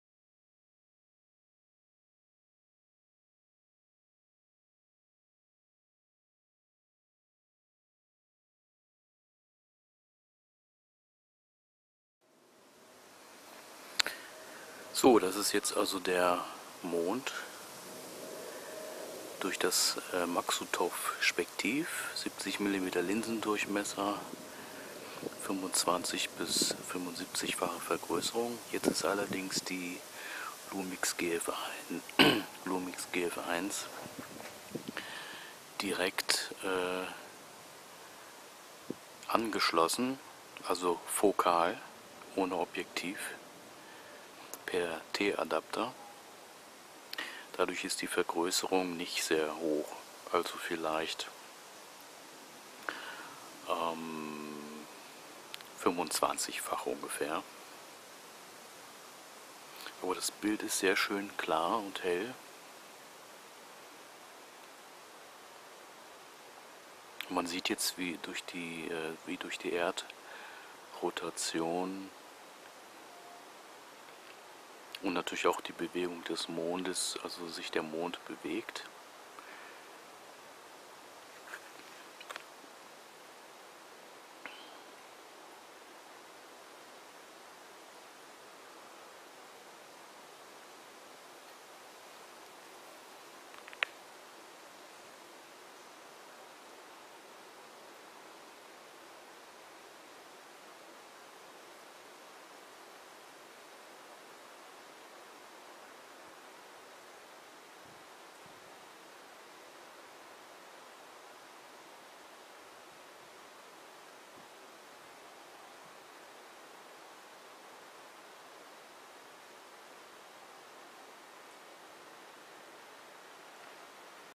Mond Test / Maksutov Zoom Spektiv Teleskop 70mm 25-75 fach mit T2 Anschluss
Eine Lumix GF1 Systemkamera per T2 Anschluss an das kleine kompakte Maksutov Spektiv/Teleskop montiert (auch TS Voyager, dann aber teurer). 70mm Linsendurchmesser, 25-75 fache Vergrößerung mit dem Zoom-Okular. Bei Ebay nur 79,90 Euro!